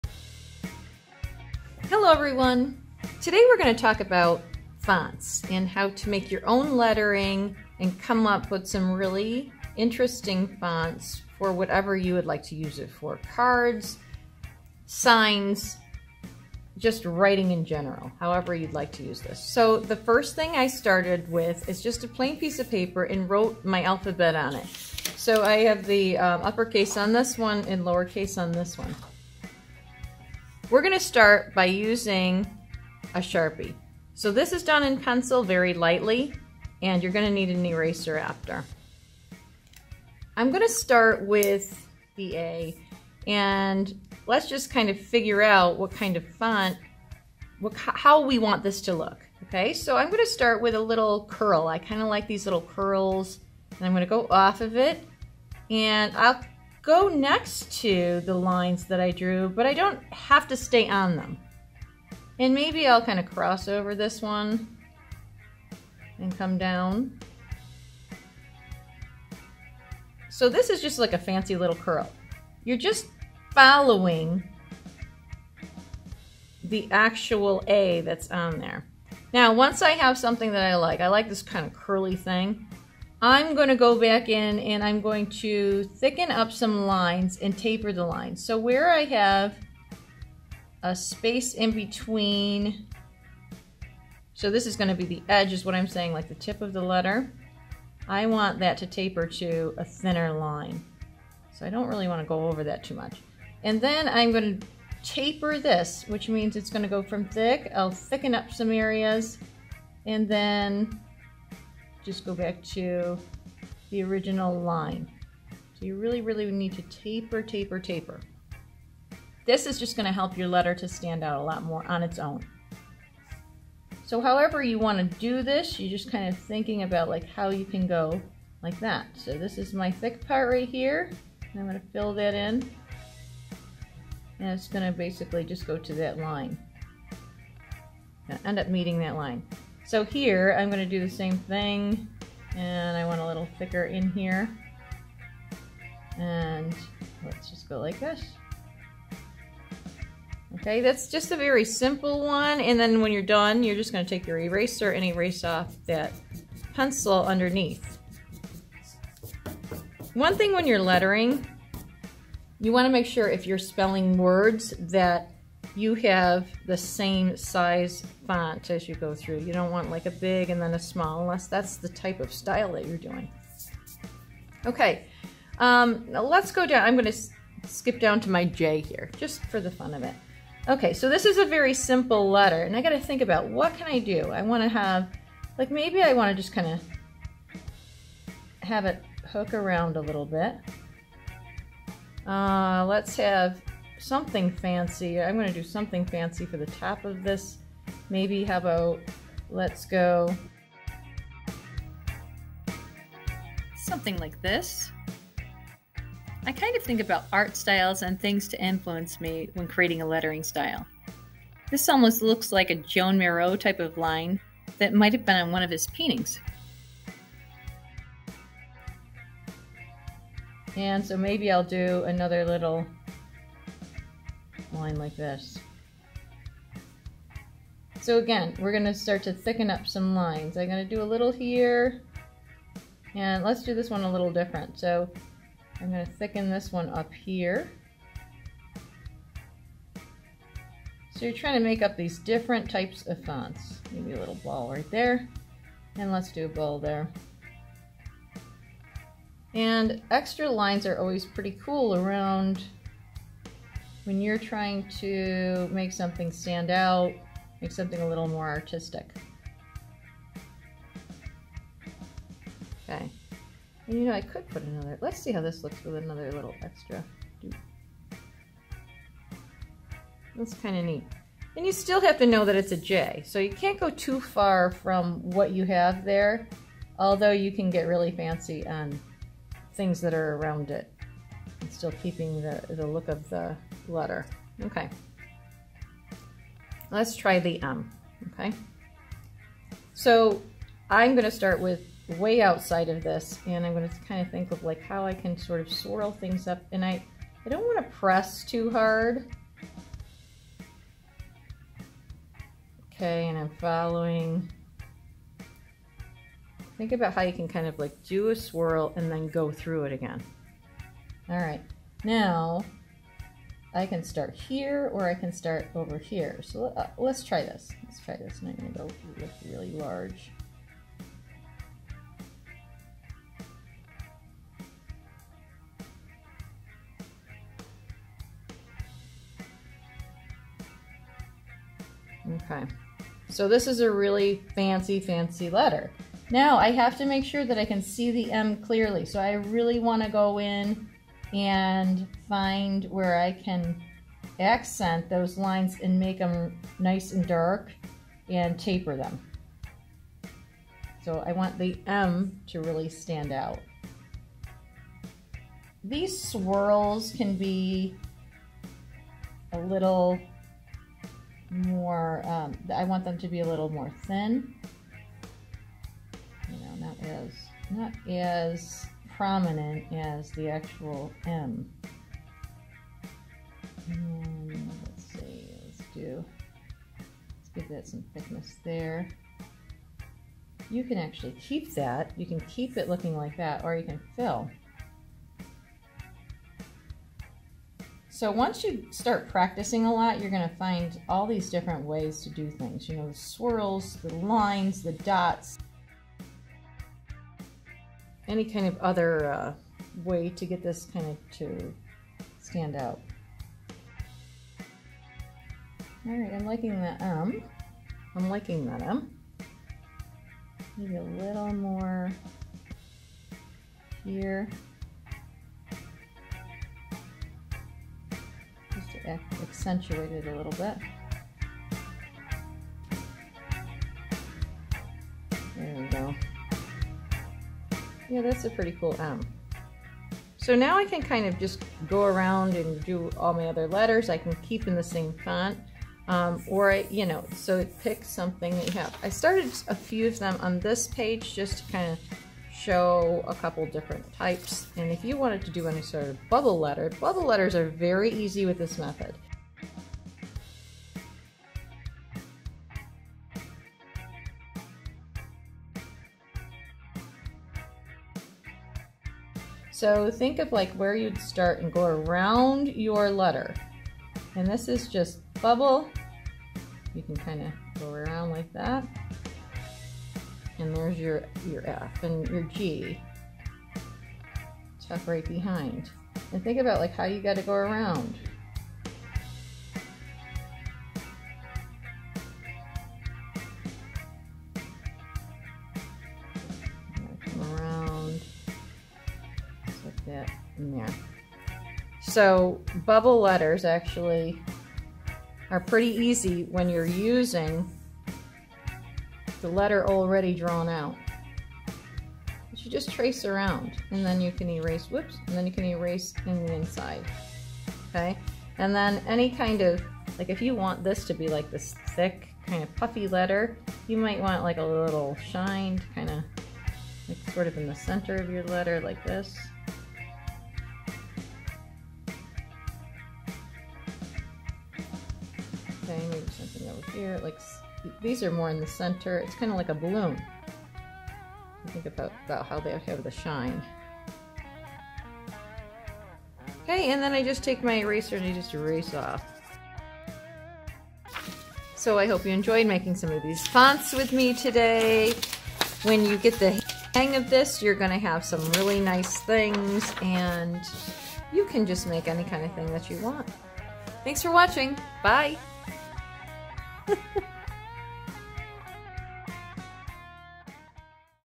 How to Create your own Lettering- lesson
Learn this easy way to create your own styles of lettering. Get ready to draw with me as I explain you through the process in real time. This will lead you to learning all sorts of calligraphy and stylized lettering for writing signs, cards, monograms, or any handwritten creation! This is a fun & useful form of art for anyone to learn.
don't forget to try the "Extra Footage" at the end - How to Create Bubble lettering!